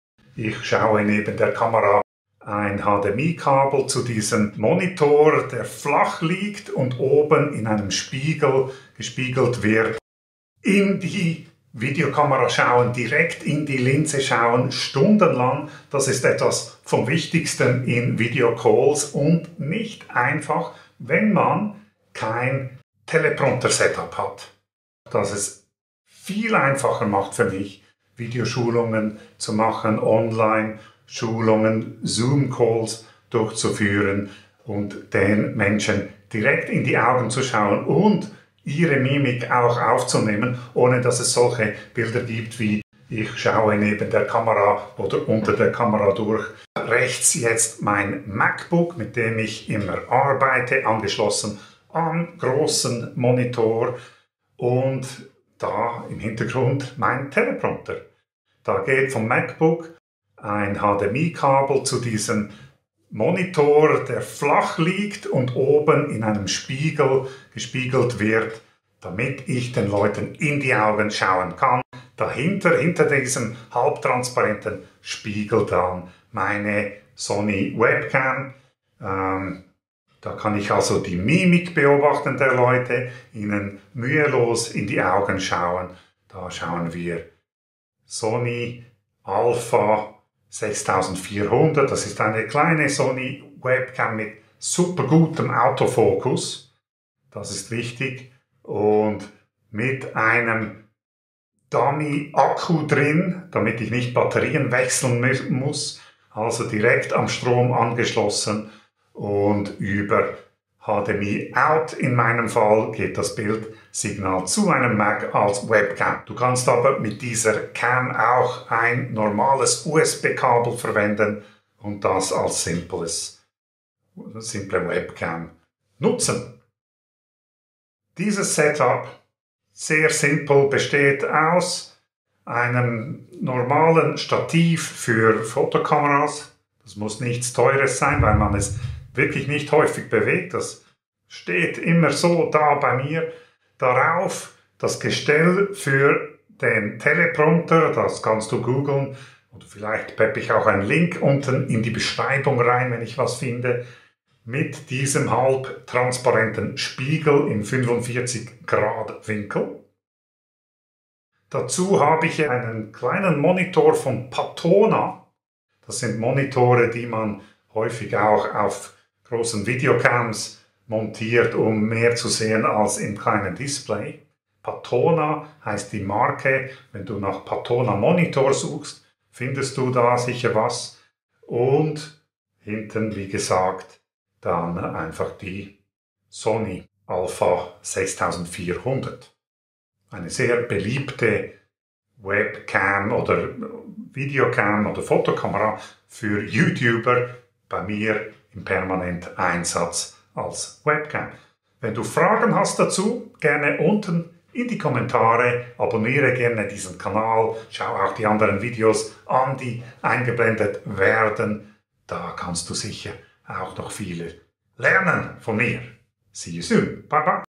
Blickkontakt in Video-Calls: Mein Teleprompter Setup erklärt - in die AUGEN schauen
Wie du in Zoom-Calls mühelos in die Kameralinse schaust und dabei auch die Mimik des Gegenübers aufnimmst. Für mich ein absoluter Game-Changer und seit Jahren im Einsatz. Möchte das nicht mehr missen. Blickkontakt in Video-Calls.

Liste der Produkte (wird laufend ergänzt):
- Velbon DV-7000N Stativ (overkill aber von meiner Frau geklaut)
- altes Teleprompter-Gestell inkl. schwarzem Tuch
- PATONA Monitor 7 (günstig und gut)
- SONY a 6400 Kamera
- Dummy-Akku für Sony (glaub von Patona)
- Mini HDMI Kabel an Elgato CamLink 4K (aus Nostalgie-Gründen: das ginge auch direkt mit dem USB-Kabel)

Ich helfe dir, online effizient neue Kunden zu gewinnen.
Damit du mehr bewirkst. Und das relaxter.